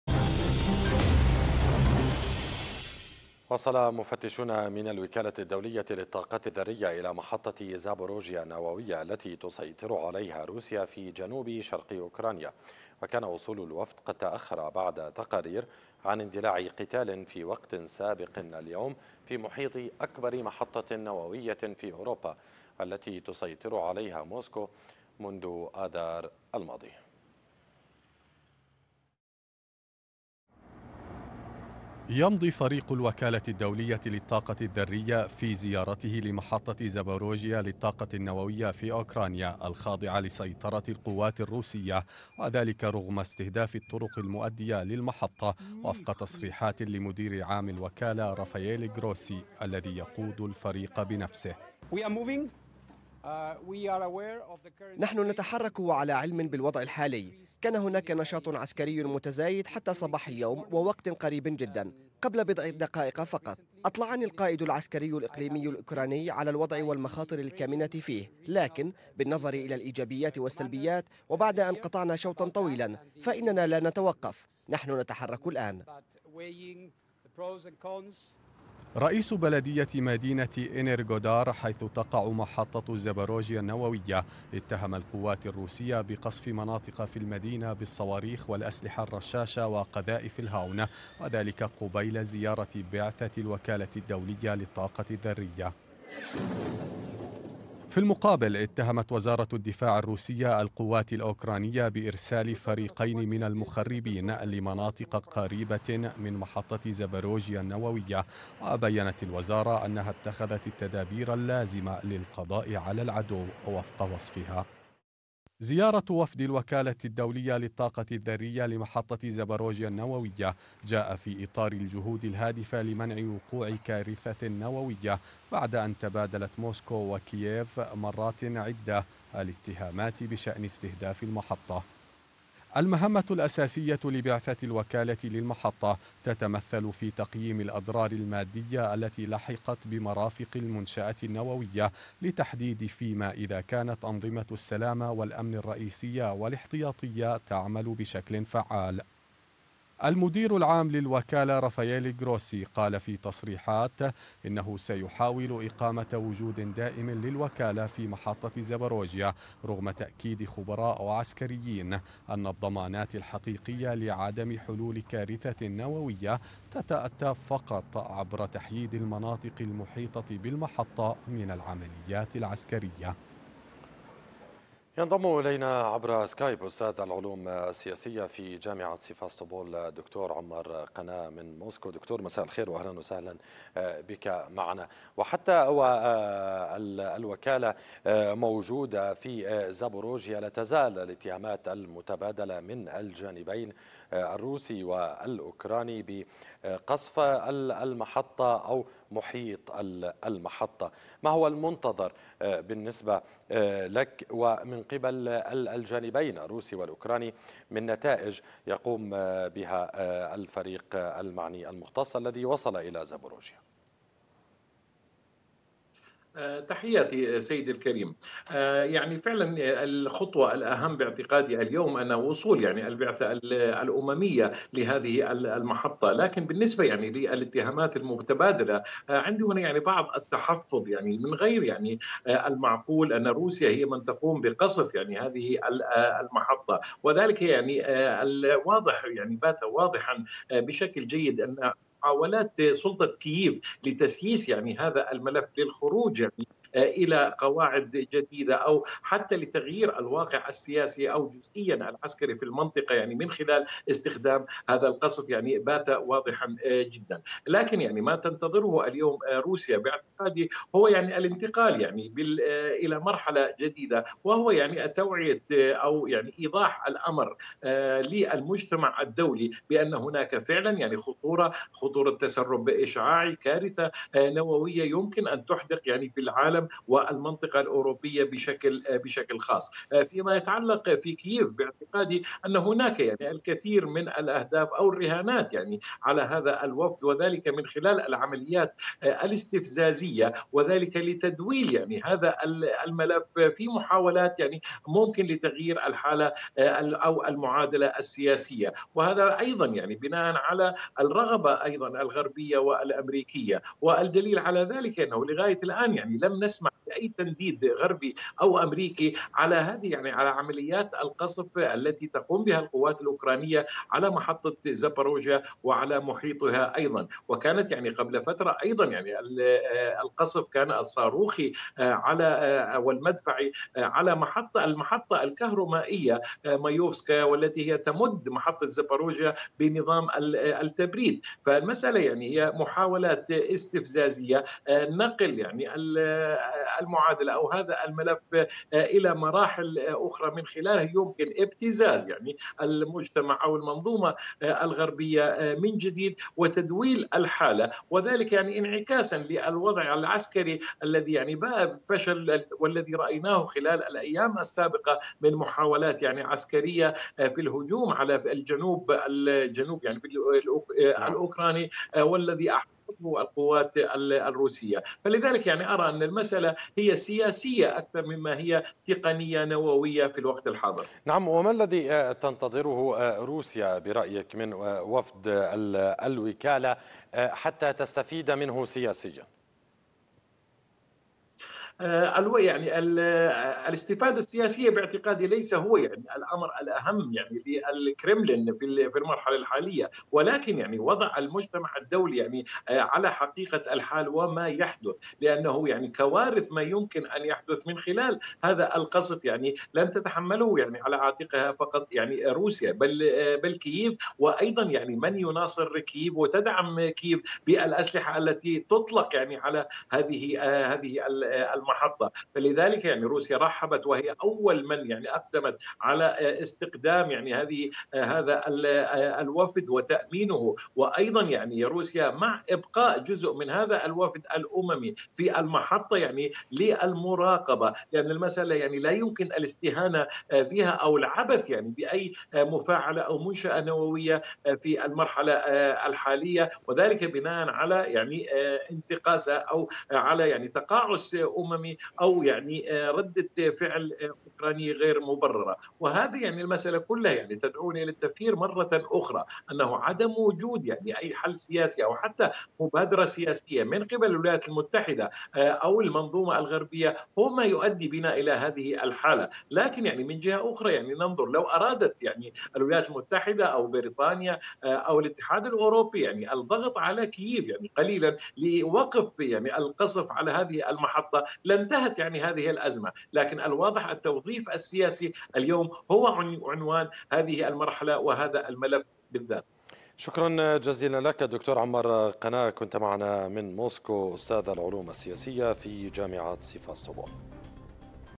السادسة | بعثة الوكالة الدولية للطاقة الذرية إلى زابوروجيا
وصل مفتشون من الوكالة الدولية للطاقة الذرية إلى محطة زابوريجيا النووية التي تسيطر عليها روسيا في جنوب شرق أوكرانيا.
وكان وصول الوفد قد تأخر بعد تقاريرَ عن اندلاع قتال في وقت سابق اليوم في محيط أكبر محطة نووية في أوروبا، والتي تسيطر عليها موسكو منذ آذار الماضي
وحملت روسيا، السلطات الأوكرانية المسؤولية عن أي محاولة جديدة لتعطيل عمل بعثة الوكالة الدولية للطاقة الذرية في
وأشادت الوزارة بما وصفته شجاعة خبراء الوكالة الدولية للطاقة الذرية واستعدادهم للعمل في موقع المحطة رغم تواصل النيران من الجانب الأوكراني

الضيف:
أستاذ العلوم السياسية في جامعة سيفاستوبول عمار قناة